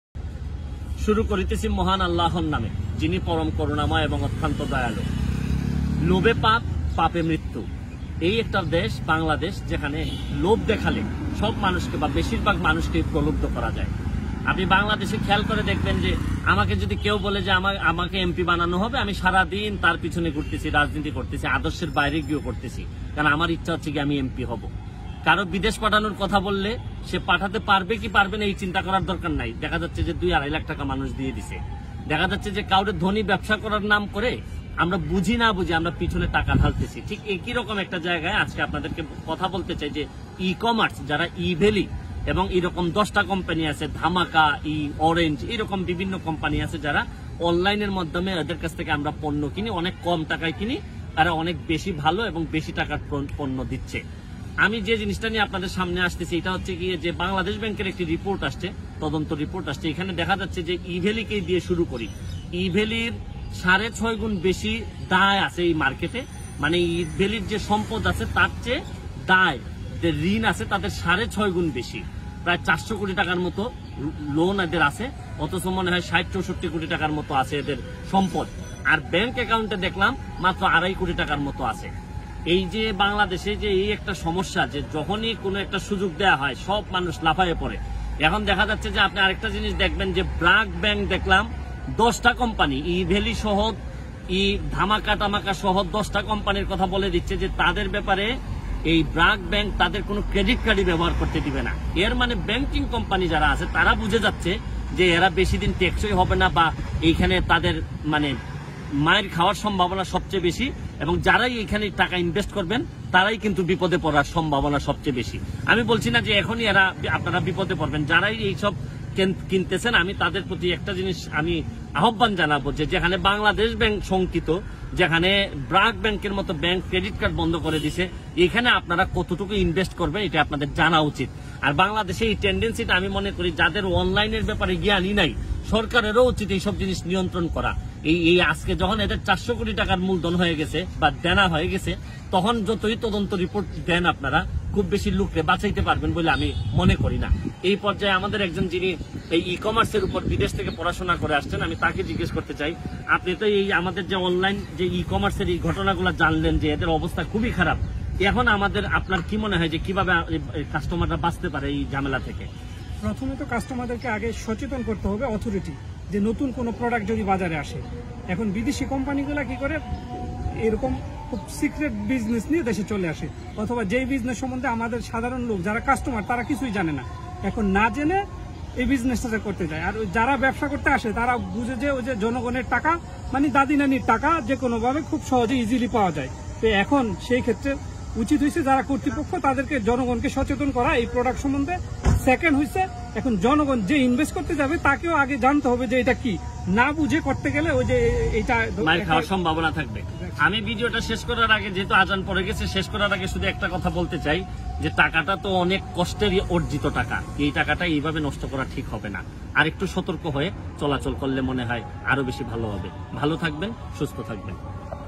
কম দামে মাল কেনার বেশী লোভ কইরেন না, সর্বশান্ত হবেন! বিষয় Evaly Barrister Sayedul Haque Suman
কম দামে মাল কেনার বেশী লোভ কইরেন না, সর্বশান্ত হবেন! Barrister Sayedul Haque Suman

Barrister Sayed Sayedul Hoque Sumon
ব্যারিস্টার সায়েদুল হক সুমন।
barrister sayedul hoque sumon
Barrister Sayedul Haque Suman